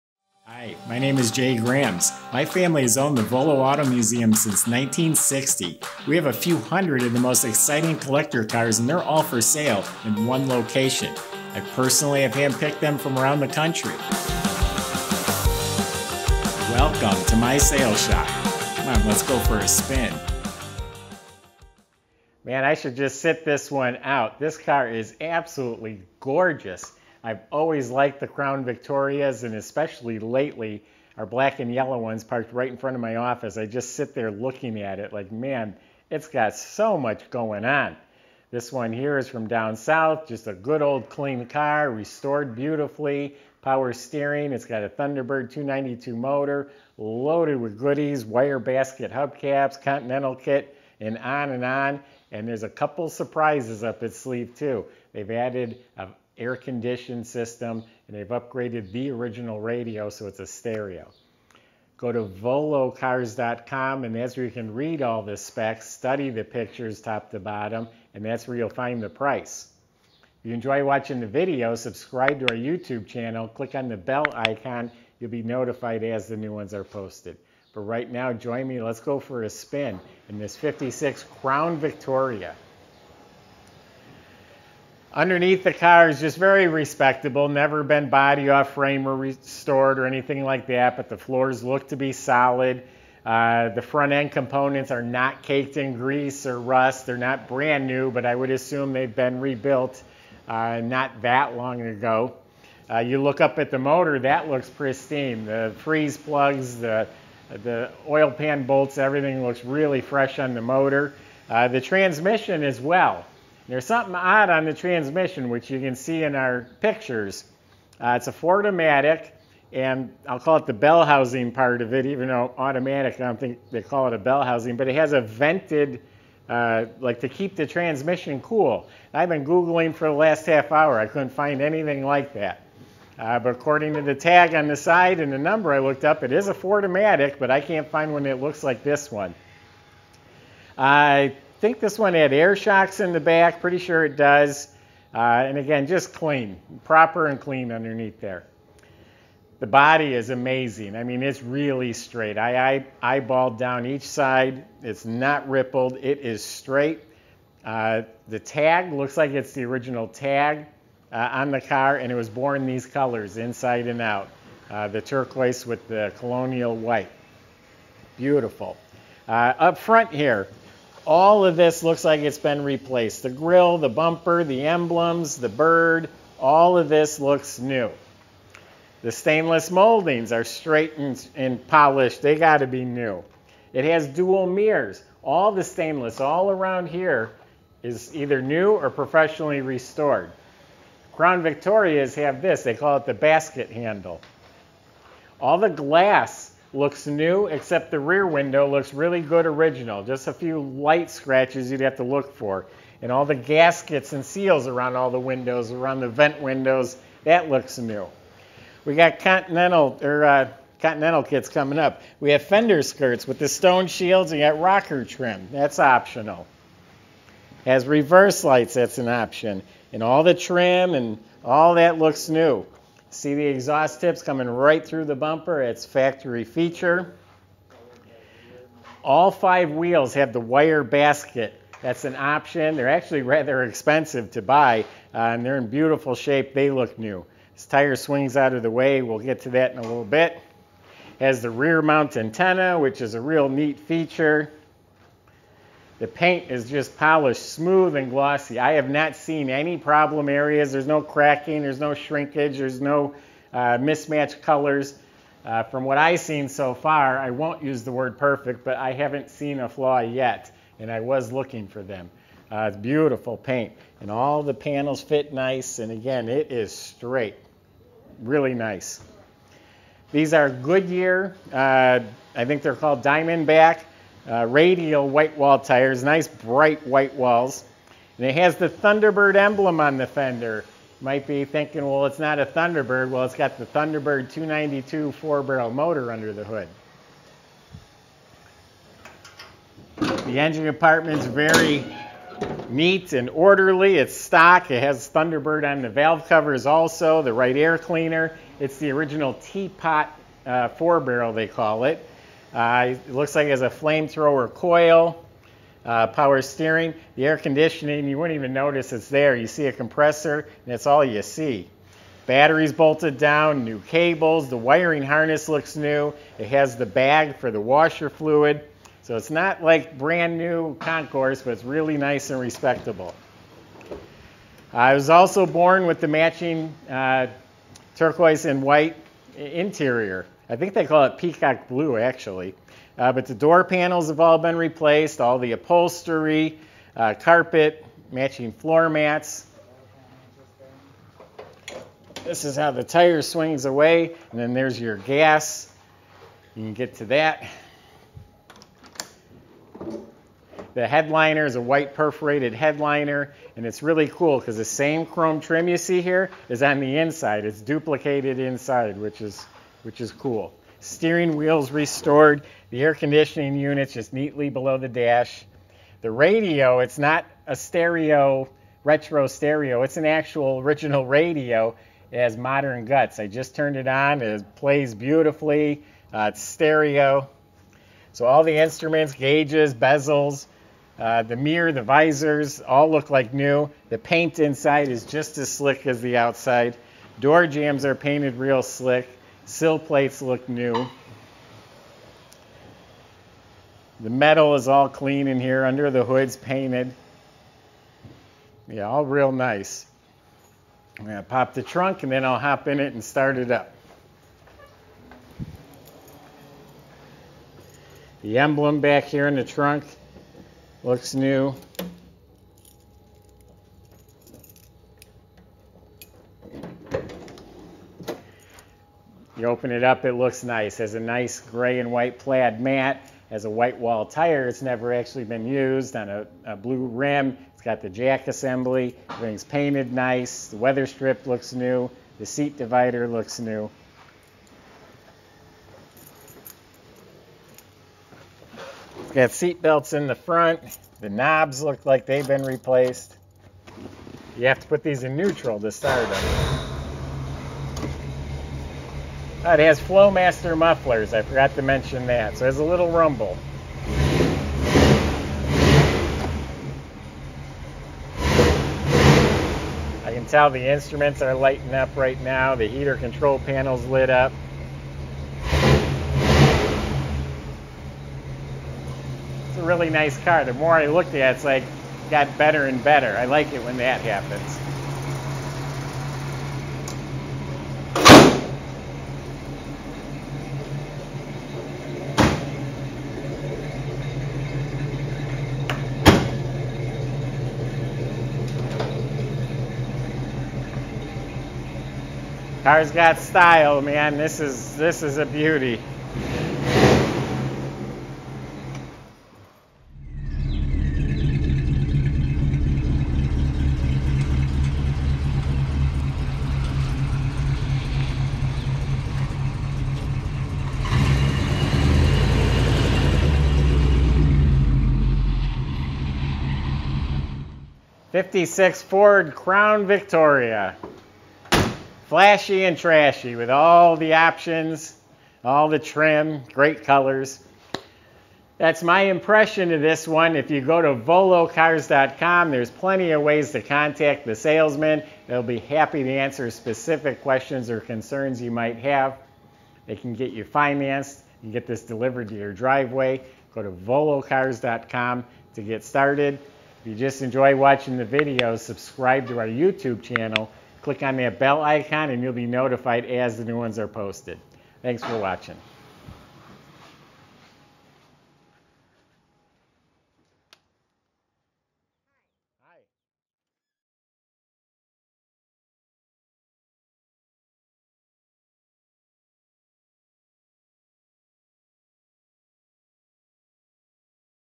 1956 Ford Fairlane Crown Victoria for sale at Volo Auto Museum (V19636)
Absolutely gorgeous. Take a second and look at the sleak lines. This one is even better with great colors and it's loaded with accessories. Easy to drive with an automatic, power steering and air conditioning. Restored to show quality.

EXTERIOR: These Crown Victoria are one of the sharpest cars of that era. The body has no rust or damage, looking down each side it is absolutely straight. The panels fit square and flush. It has optional fender skirts and a painted continental kit. It is repainted in it's original colors- Peacock blue and colonial white. The paint is very smooth and glossy. Upfront all the chrome is excellent. The Thunderbird emblems on the fenders designate the special motor that's under the hood. The stainless moldings running down the sides, around the windows and the basket handle across the roof are straight and polished. The accessory dual mirrors, rocker moldings, stone shields and rear antenna looks like new. In back the tail lights, trim and bumper look new. The dual exhaust tips come right through the bumper. All the glass looks new except rear window which is excellent. New gaskets and weatherstrips. It rolls on Goodyear radial white wall tires with optional basket style wire hubcaps.

INTERIOR: It is really beautiful inside this car. Matching turquoise and white like the exterior. The dash in front of me is gorgeous, all the metal trim inside is painted smooth and glossy. The knobs, chrome and steering wheel look new. The original radio was sent out and fitted with modern internals making it an Fm stereo. An air conditioner is neatly mounted below the dash. Down on the floor it has sound deadener and turquoise carpet with matching floor mats. The seats are reupholstered white with the turquoise fabric inserts. Door panels are replaced with reproduction. The visors and headliner were replaced. It has the same chrome trim inside as on the roff. The glove box liner is new, kick panels are new.

ENGINE COMPARTMENT: It has the Thunderbird Special motor under the hood. A 292-202 hp with original tea pot 4 barrel carb. It features power steering and air conditioning has been neatly added. The 12 volt battery is bolted down properly. Everything looks neat, clean and orderly. The wiring harness looks like it's been replaced.